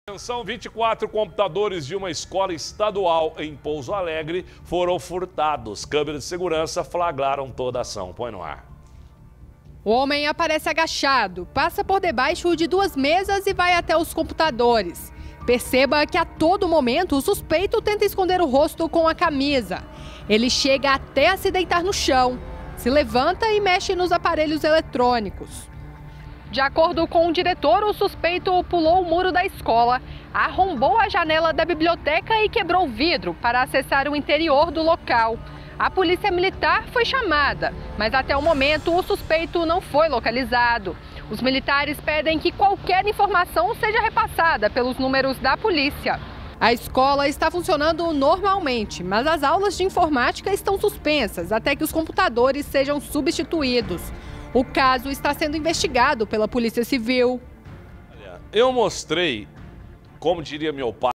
Furto: 24 computadores de escola são levados em Pouso Alegre
FURTO: 24 COMPUTADORES DE ESCOLA SÃO LEVADOS | Um homem foi flagrado durante furto em escola em Pouso Alegre. Segundo a polícia, 24 computadores foram levados.